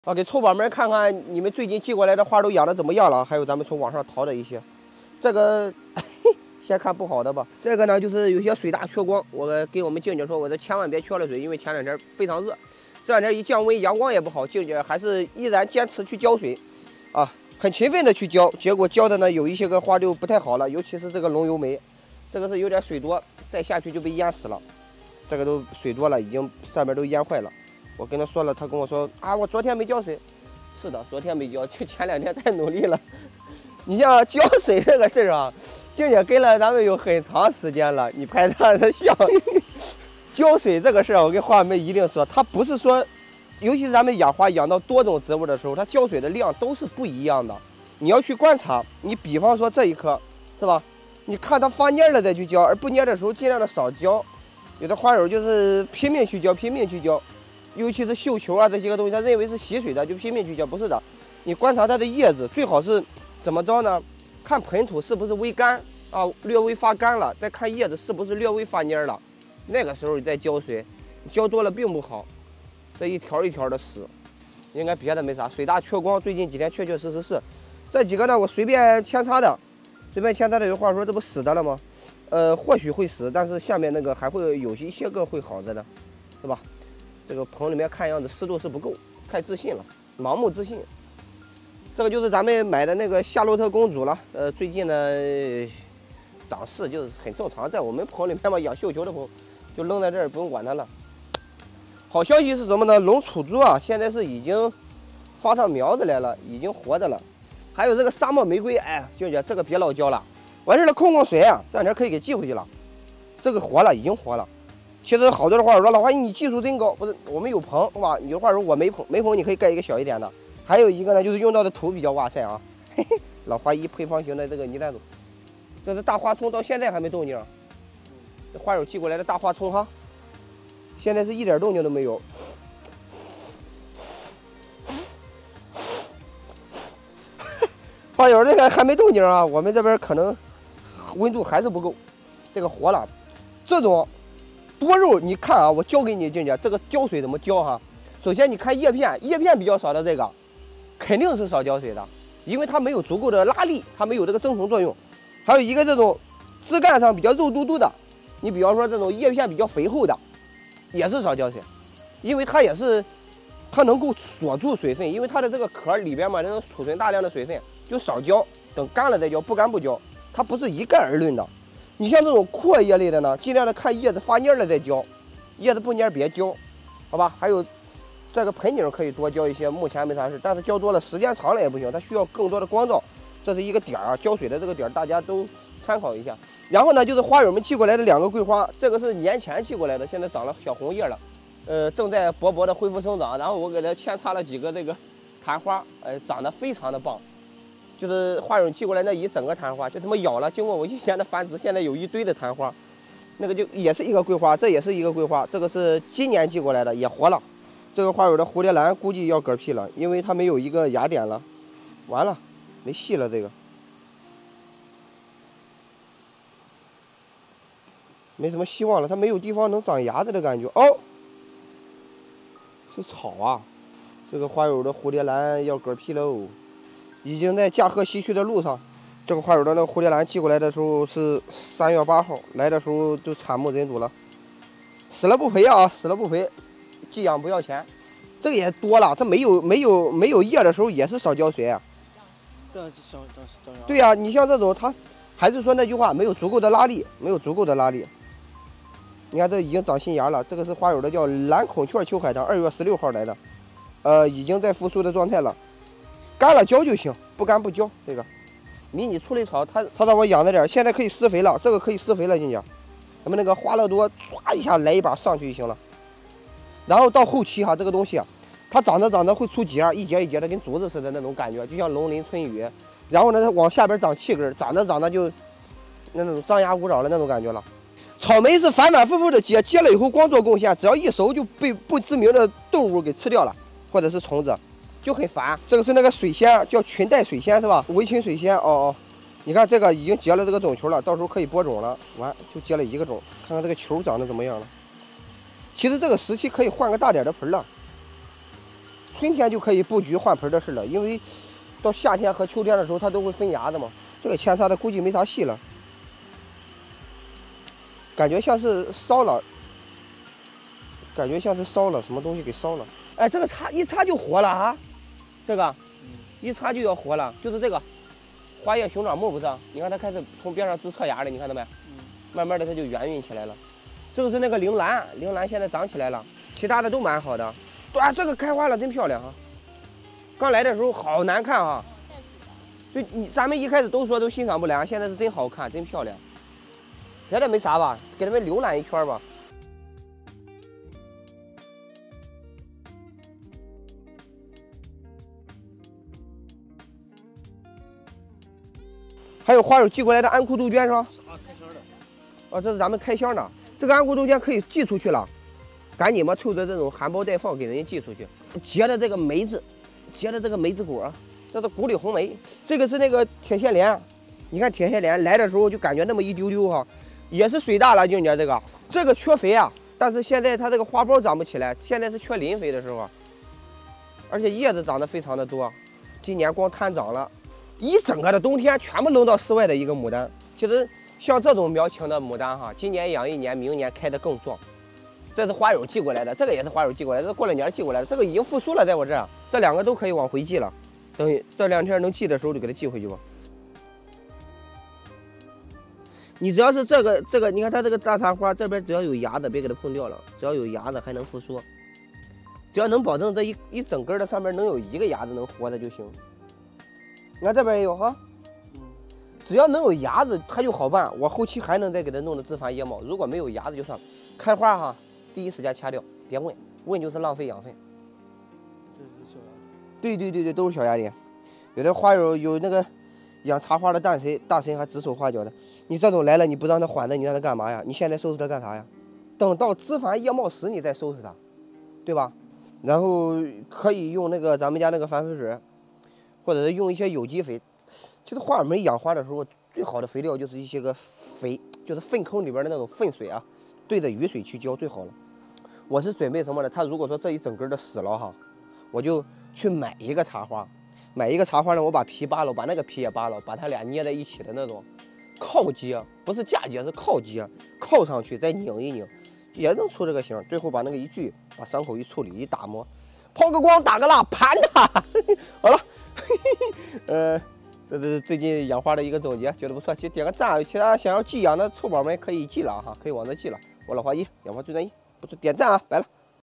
【寄养总结】最近养的花怎么样了？不同状态的盆栽如何区别浇水？寄养总结 How to water different plants? Summary of rescuning plants.
龙游梅水大了？牡丹需要施肥了？扦插苗有活有S？老花医寄养总结
How to water different plants? Summary of rescuning plants.